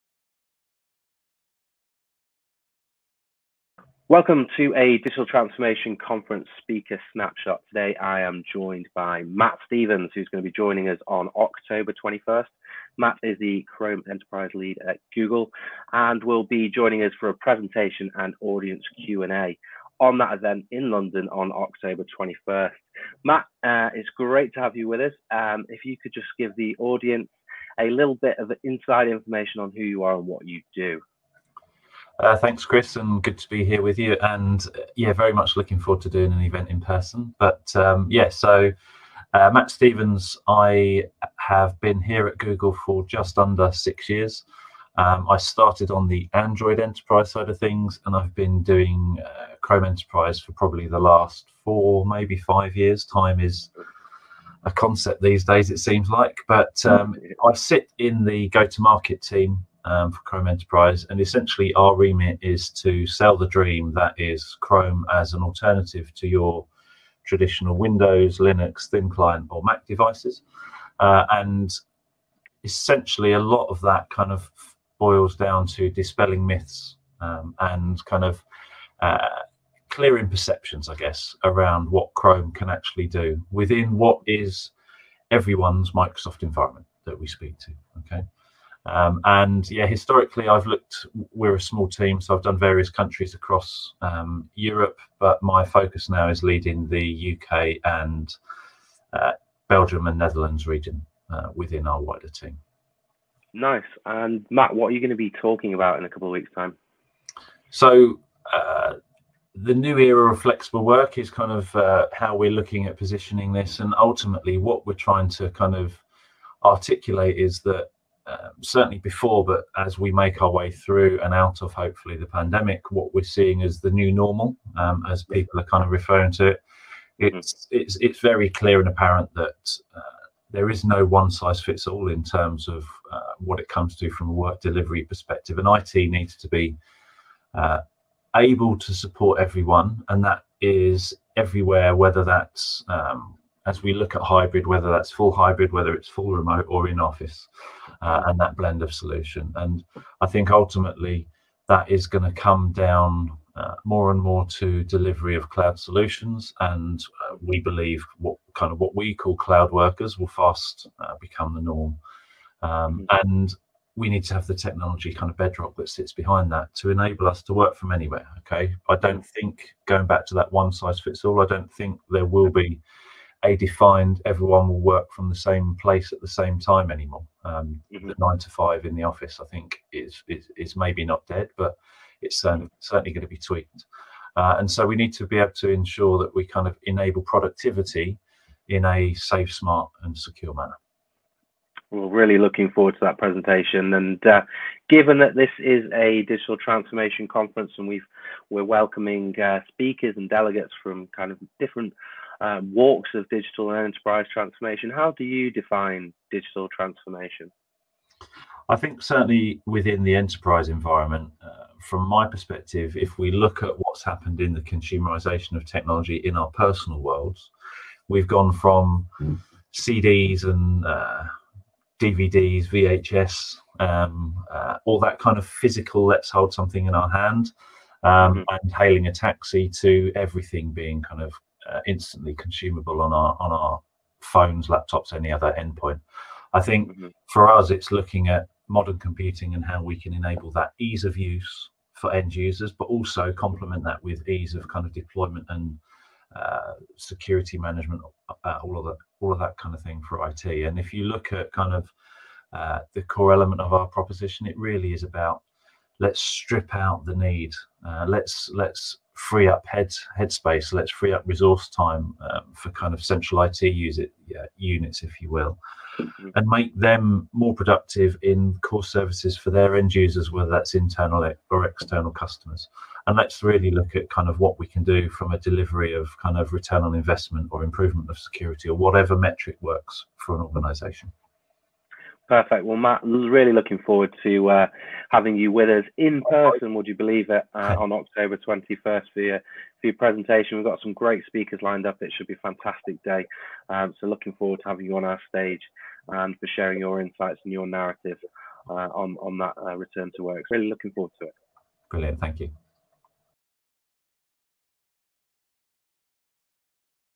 Speaker Snapshot: Matt Stevens, Chrome Enterprise Lead for the Digital Transformation Conference
Matt Stevens, Chrome Enterprise Lead at Google joins the speaker line up for the London Digital Transformation Conference taking place on October 21st with a keynote presentation; Welcome to the Era of Flexible Work, joining speakers from BT, Legal & General, Natural History Museum, NatWest & plenty more. In this short speaker snapshot Chris Towers, Head of Events at Roar Media, had a quick catch up to see what the presentation would entail! Watch below and don't forget to take a look at the line-up & more